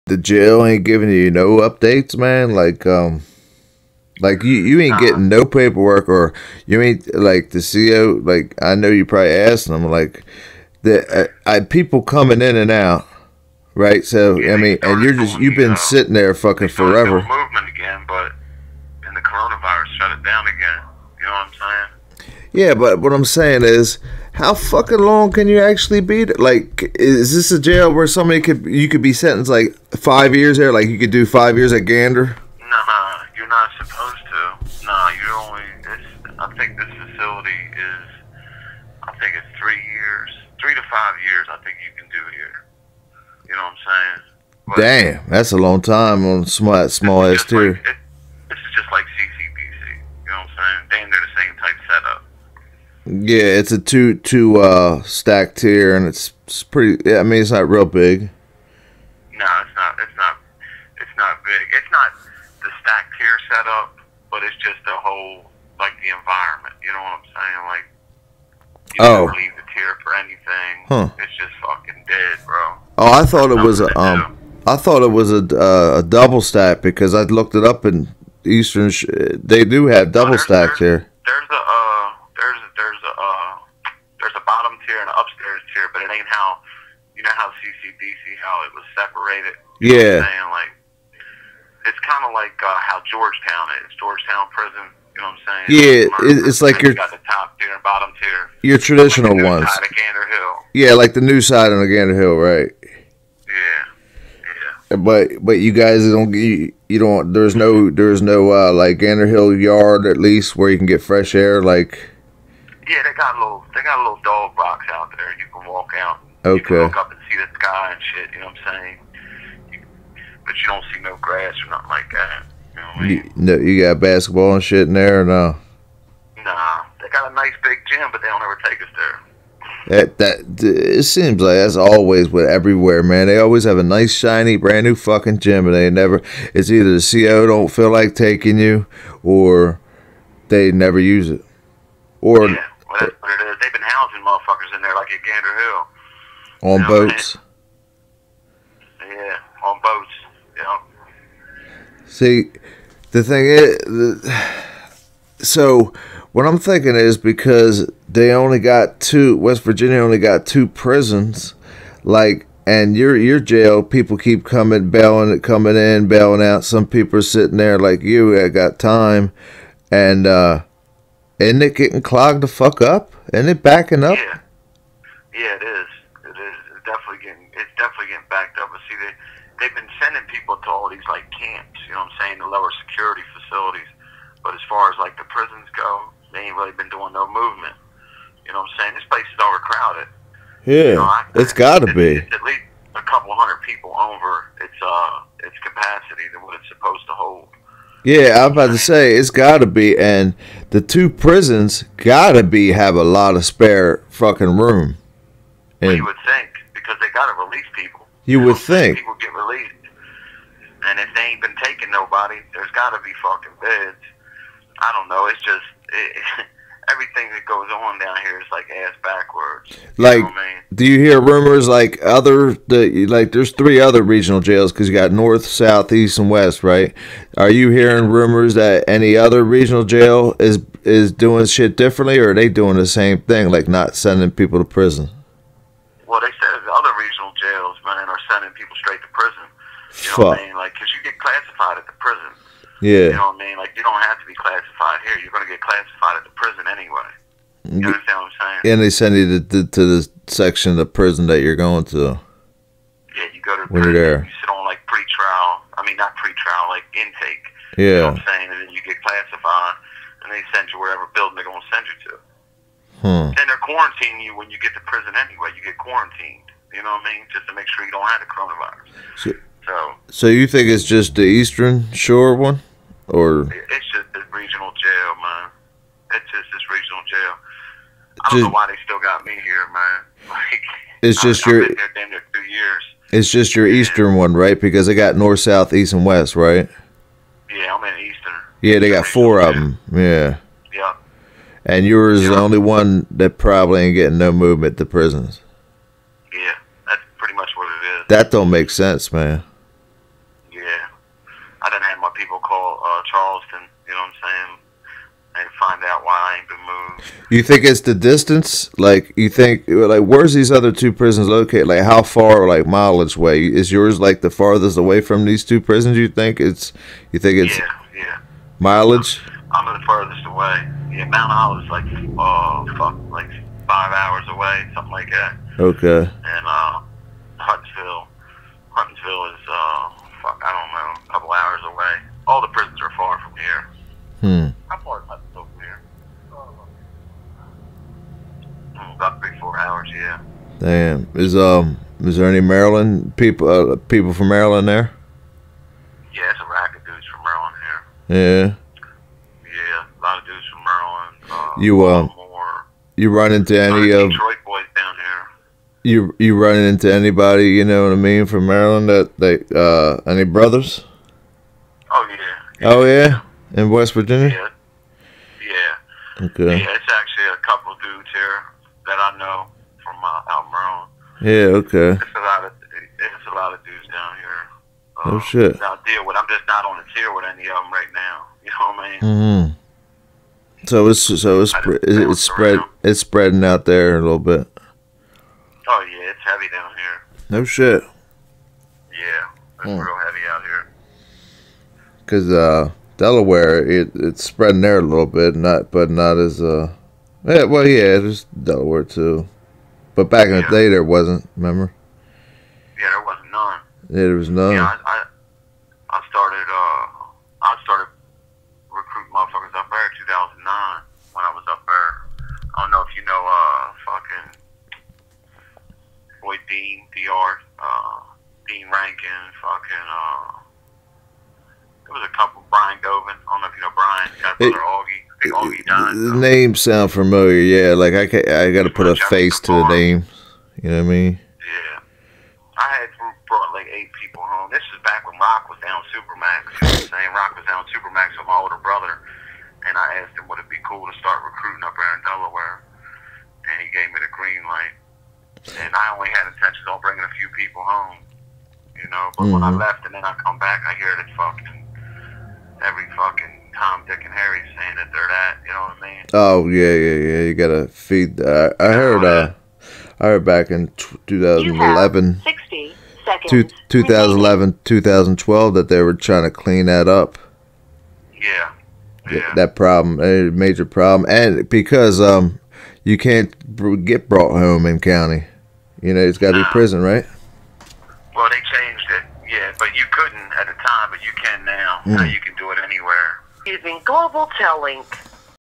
West Virginia Prison's, Prison Gangs, 'Kenny' Part 16
In Kenny Part 16 He's very frustrated that this new virus "Variant" supposedly put all transfers to West Virginia prisons until further notice which is bullshit because Kenny has been sitting at the Eastern Reginal Jail in WV for over 2 years now! He also talks about his affiliation with his brothers locked up and when he gets sent to one of the 2 prisons the state of West Virginia has he'll at least be able to unite with some of them since the State of West Virginia is right next to the State of Maryland which is the birthplace of the DOG'S🙌